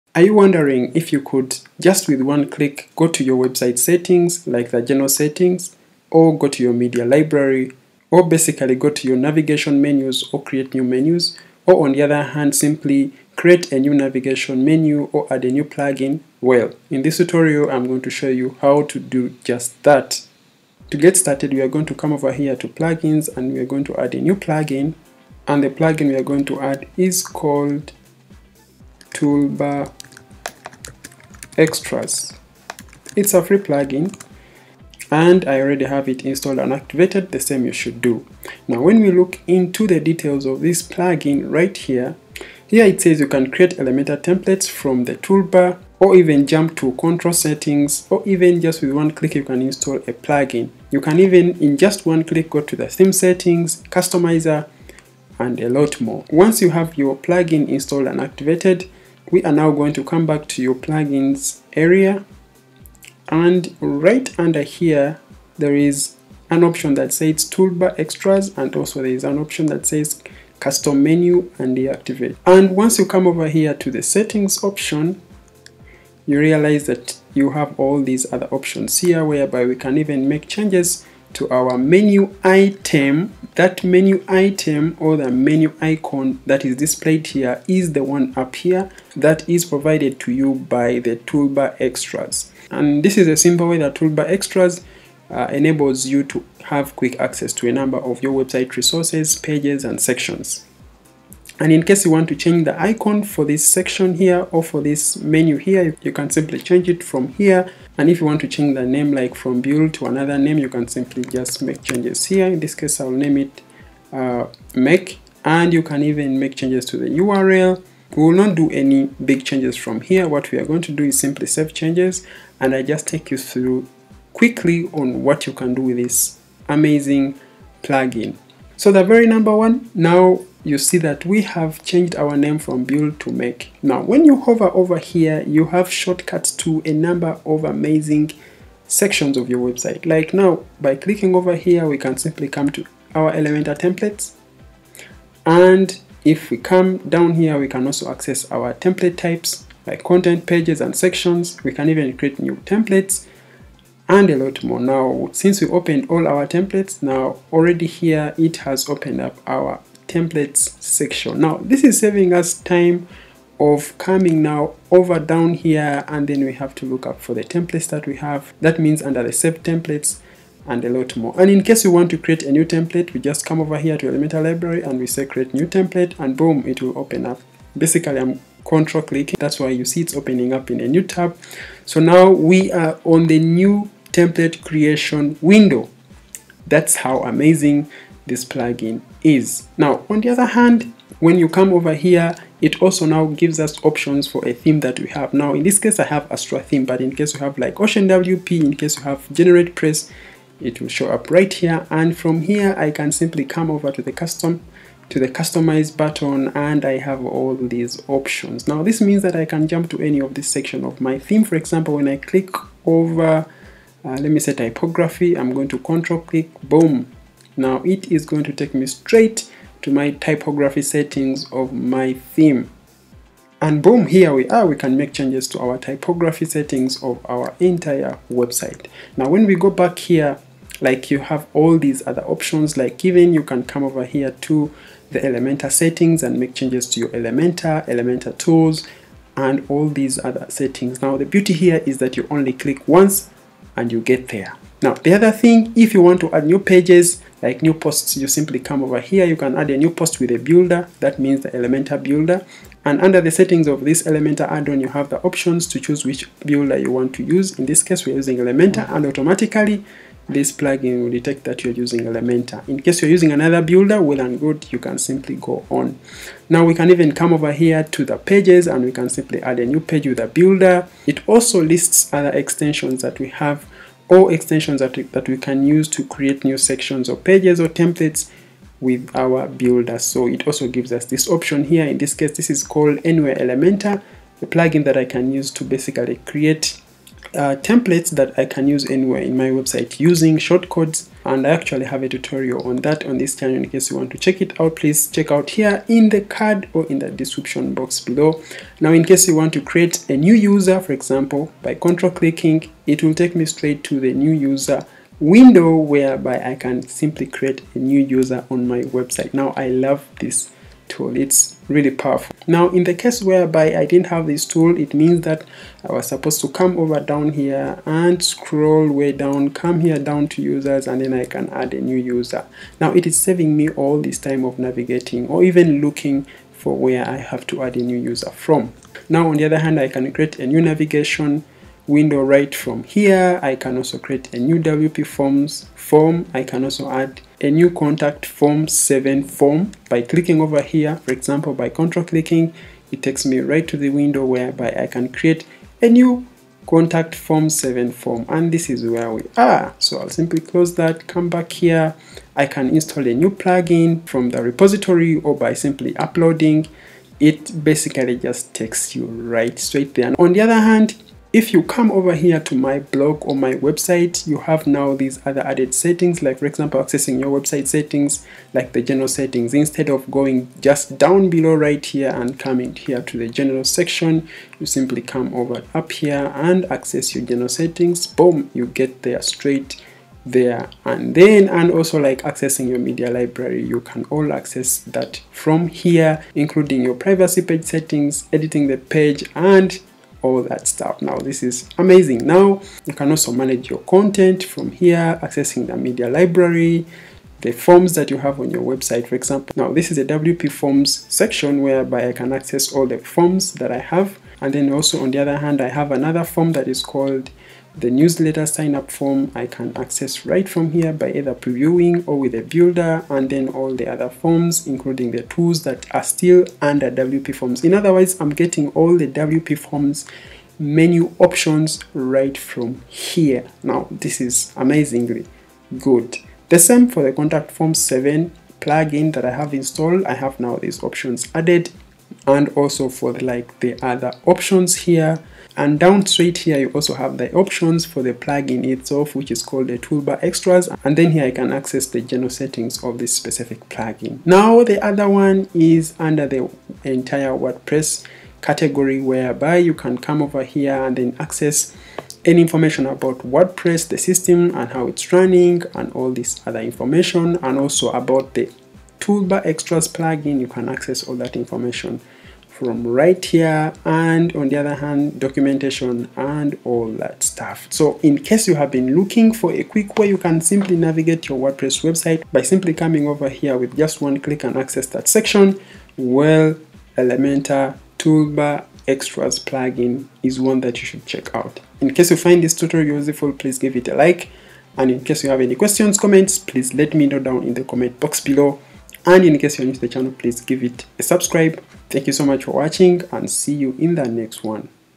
Toolbar Extras for Elementor | Quick Access To Pages and Settings in WordPress Admin Bar 🔥🔔🔥
Get quick access to your website's content, pages, settings, plugins  and themes in just one click with toolbar extras. Toolbar extras is an extended plugin that makes your work easier and faster to edit and access your website resources. It's such a big time saver and FREE. You can create elementor templates, upload new plugins, access site settings with one click on the toolbar. Check out this video as i take you through.